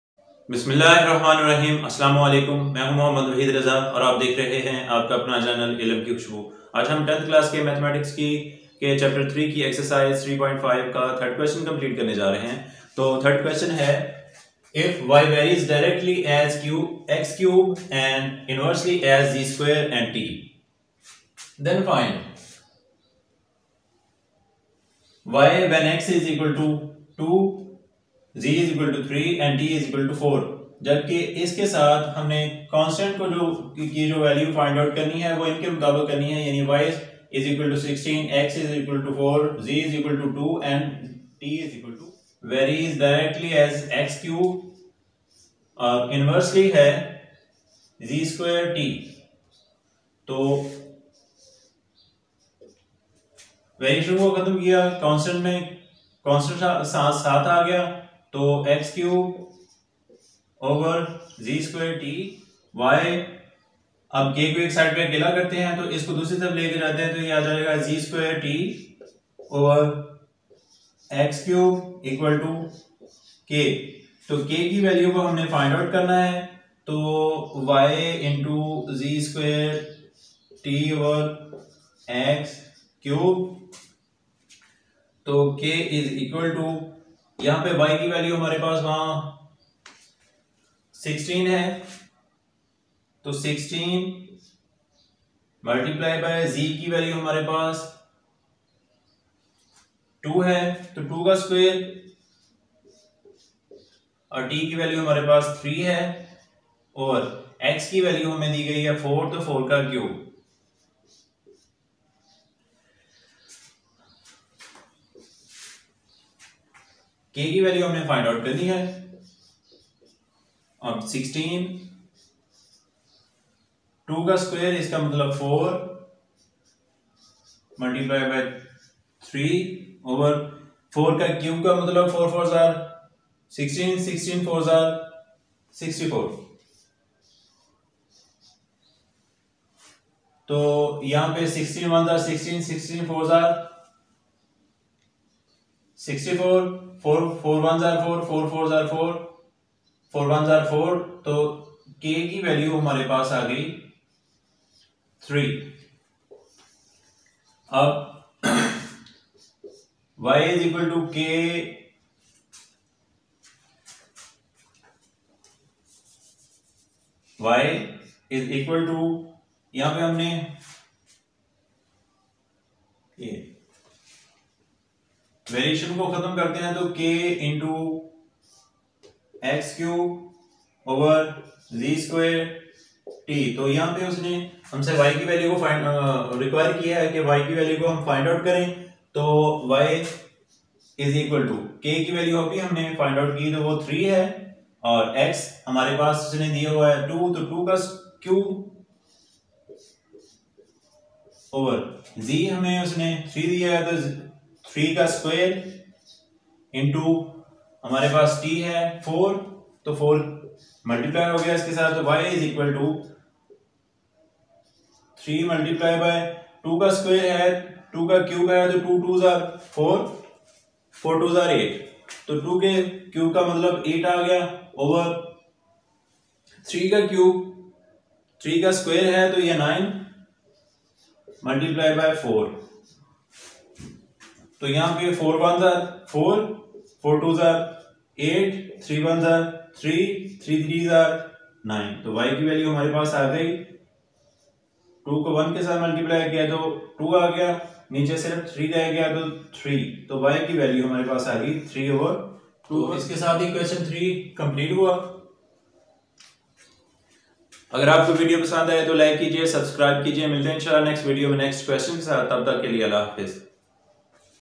10th Class Math Solution Chapter 3 Exercise 3.5 Question 3 |waheed raza| ilm ki khushbo| 1080p
ilm ki khushbo is a channel which is serving you with knowledge and education.
In this video waheed Raza is teaching and guiding how to solve the questions of 10th class mathematics chapter 3 exercise 3.5
All the students of 10th class can get the guidance by watching this video. Students can get guidance not only for the specific question but they can get guidance for all questions of the specific exercise with the help of this video.